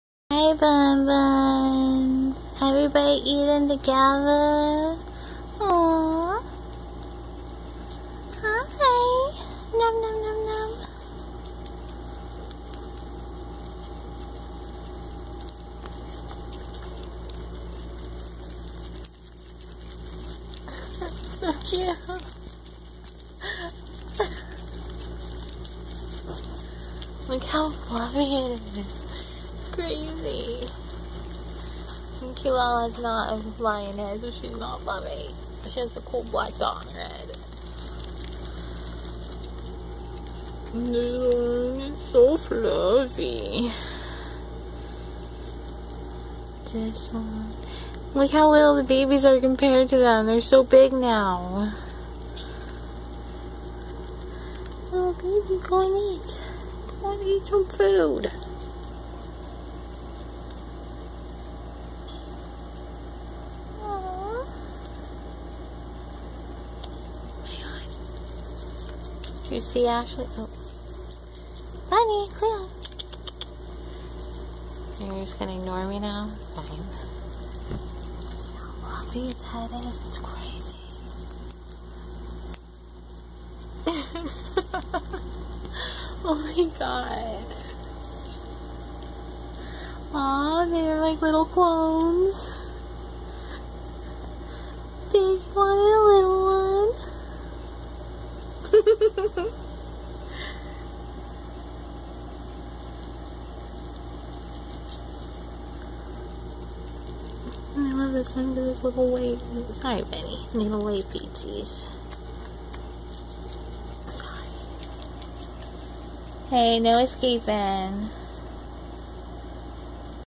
num nums baby bunnies eating
All the babies sharing a snack, the older siblings and the younger ones.